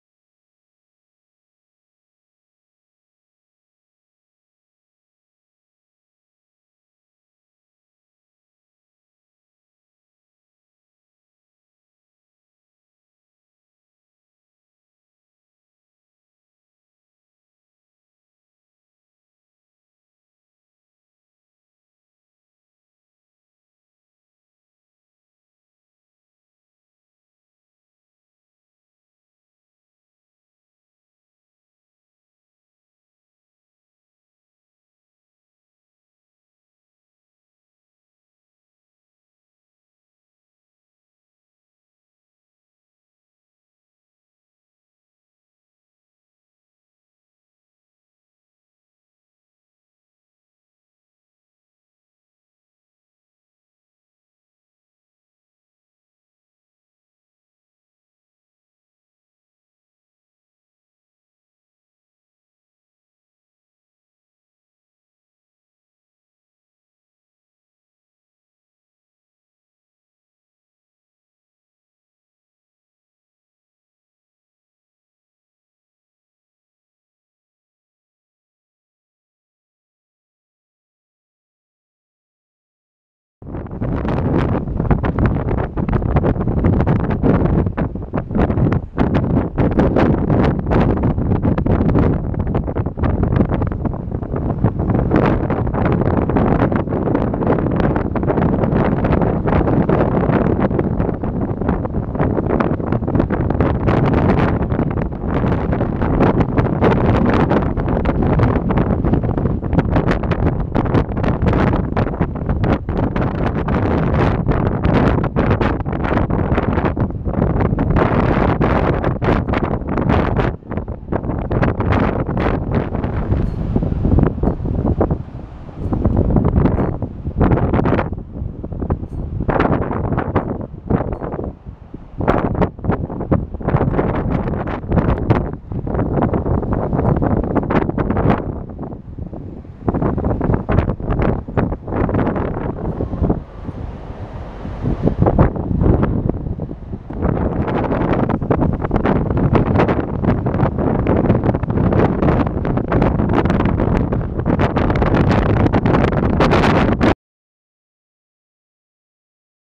INFOMIÑO TEMPORAL A GUARDA 9 ENERO 2016
Escuma do Mar sobre a estrada no Porto de A Guarda - 9 de Xaneiro de 2016